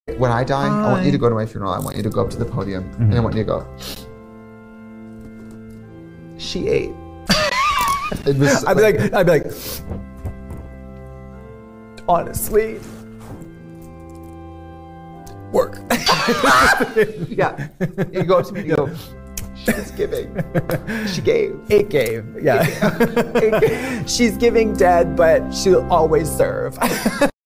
💅✨ Mettaton’s FUNeral [Skit] ~ UT/DR + Gacha Life 2 ✨💅
They're so slay 😩💅✨
Extremely proud of how MTT and Queen turned out in GL2 lol
(Damb I'm on an upload spree ngl)

Made with:
Gacha Life 2
CapCut

Characters are from Undertale and Deltarune by Toby Fox

Have a nice day y'all ✨